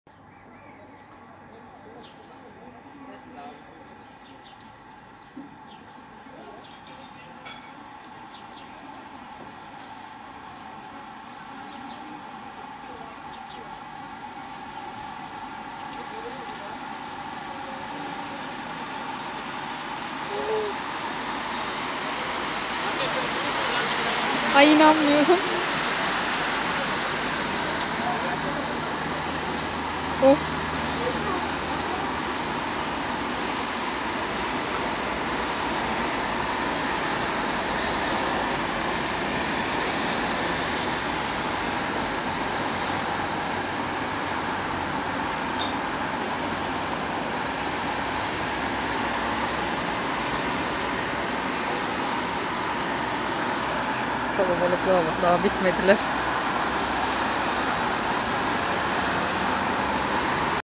2021 ULUSLARARASI YOL BİSİKLET YARIŞLARI | Alanya
2021 ULUSLARARASI YOL BİSİKLET YARIŞLARI BAŞLIYOR
Grand Prix Alanya Yol Bisiklet Yarışı 6 Şubat Cumartesi günü 18 ülkeden 176 sporcunun katılımı ile gerçekleştirildi.